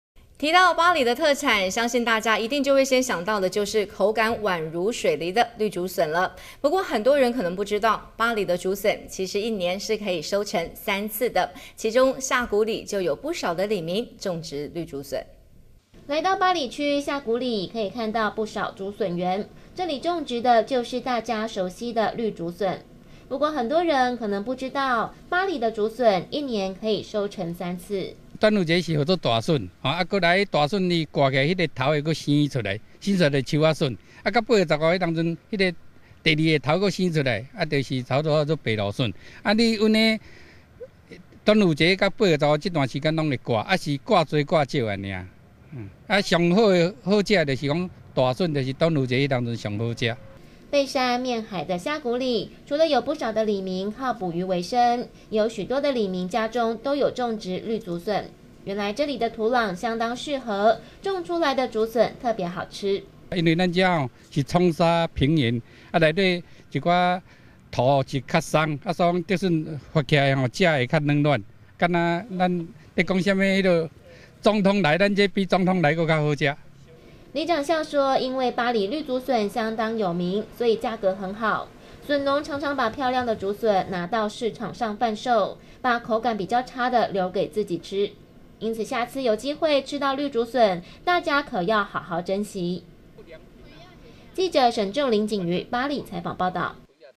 NTCnews_20160801- 09八里綠竹筍 每年生產三次
提到八里特產，大家會先想到口感宛如水梨的綠竹筍，不過很多人可能不知道，八里的竹筍，其實一年可以收成三次，其中下罟里就有不少里民種植綠竹筍。